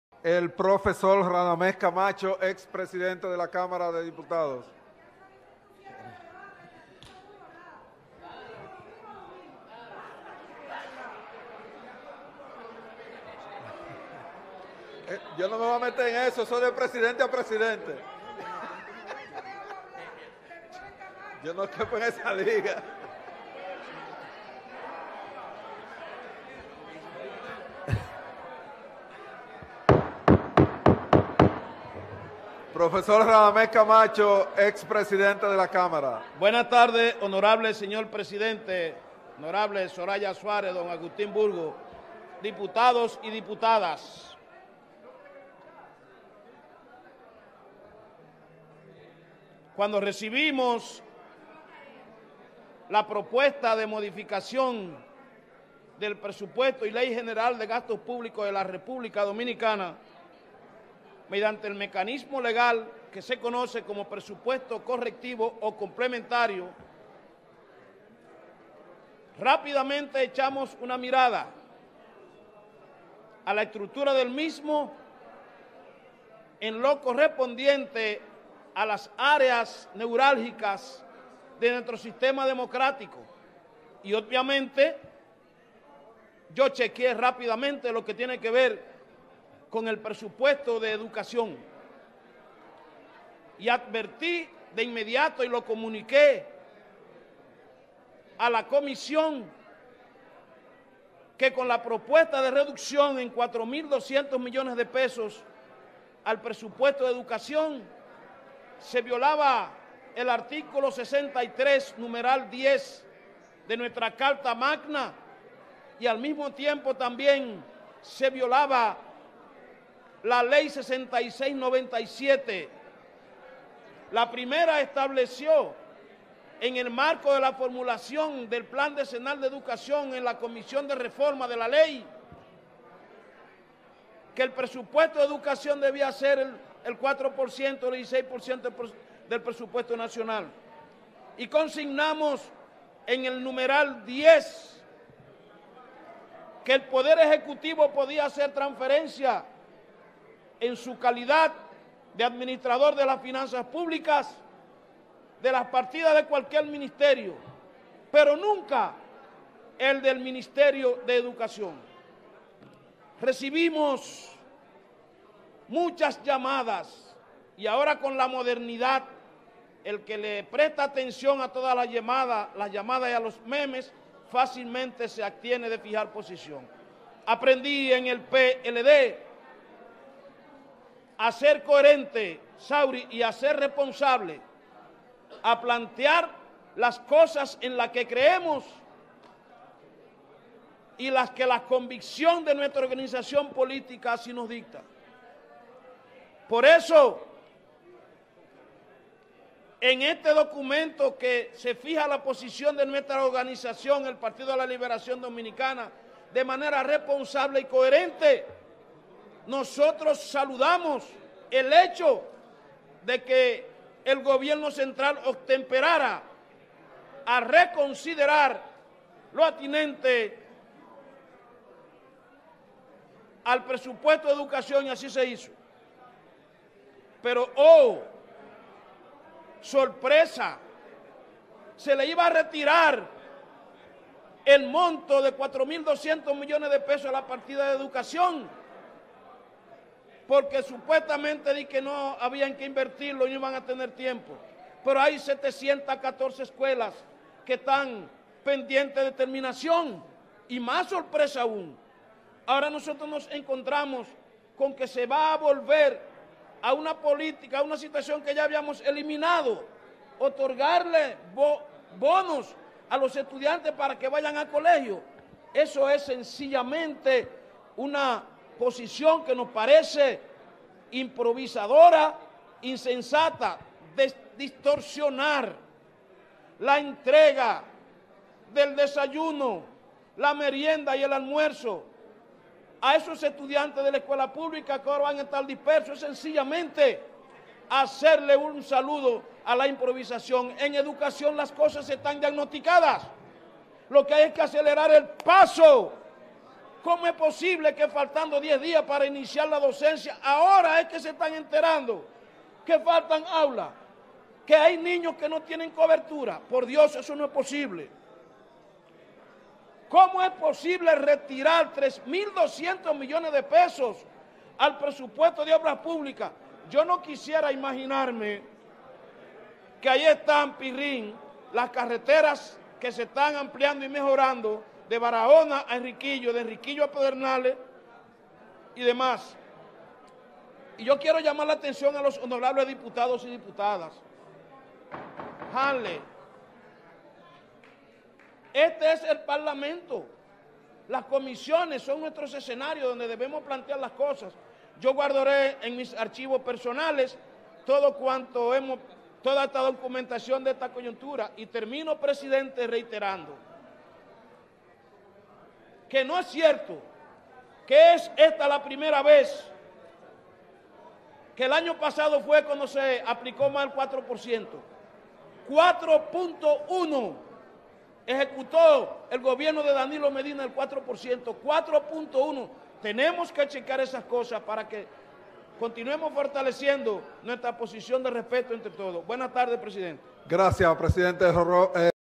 Turno del Diputado Radhames Camacho, Sesión No. 0008, martes 0008 Sep. 2022
El profesor @ sostiene que le informo a la comisión los artículos y la ley que se violaban al intentar reducir la partida de educación. El Poder Ejecutivo puede hacer transferencia   pero nunca de @educacionrd Actualmente 714 escuelas están pendientes de terminación.